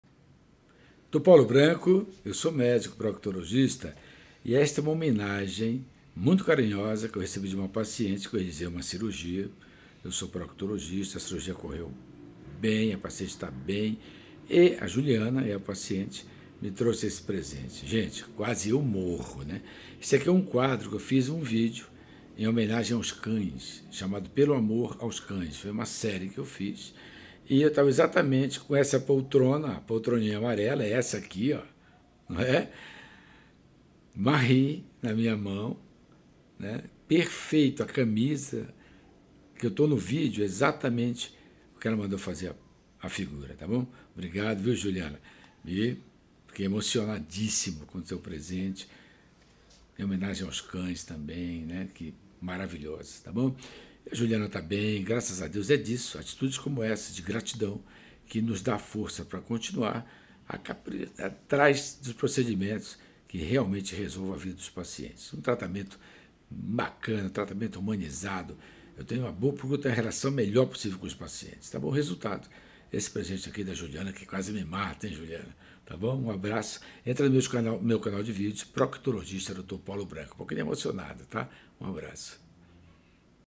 Paciente faz homenagem Dr Paulo Branco envolvendo os cães fiquei muito emocionado.
Fiquei muito emocionado e feliz pela homenagem recebida da paciente juliana que envolveu de forma carinhosa os cães que amo tando.
OBRIGADO DE TODO CORAÇÃO:
Dr Paulo Branco.
YouTuber: proctologista dr paulo branco